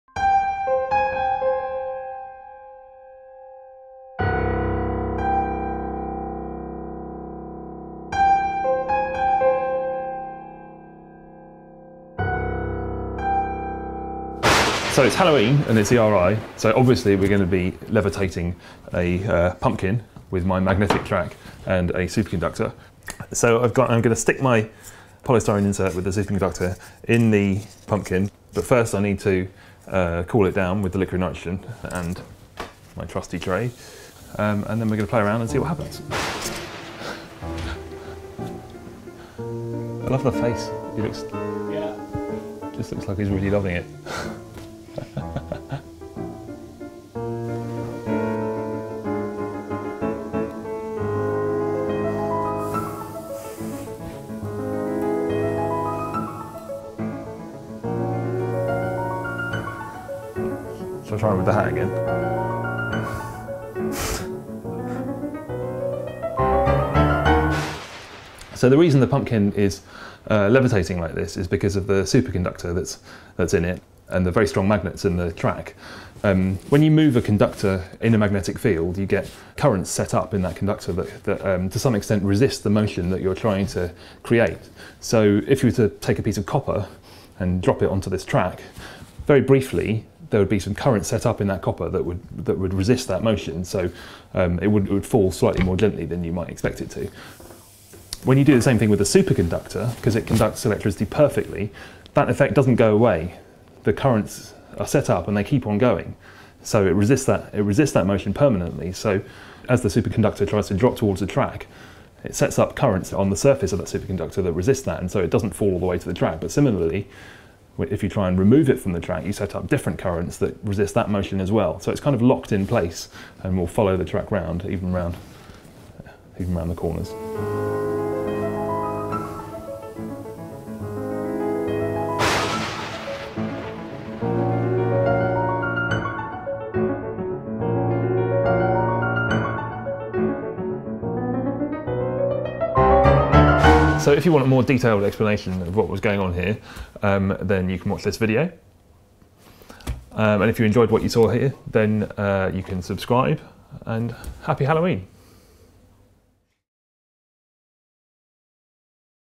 Levitating pumpkins! Halloween science
"And the cursed pumpkin rose up from the earth, carried to the sky by the spirits of All Hallows' Eve." Or in other words, Andy put a superconductor in a pumpkin and levitated it on a magnetic track.

Happy Halloween! To celebrate this year we thought we’d have some fun with a pumpkin and superconductor. Inspired by this previous video Andy took to the prep room to imbue a cheerful pumpkin with the eerie powers of superconductor levitation.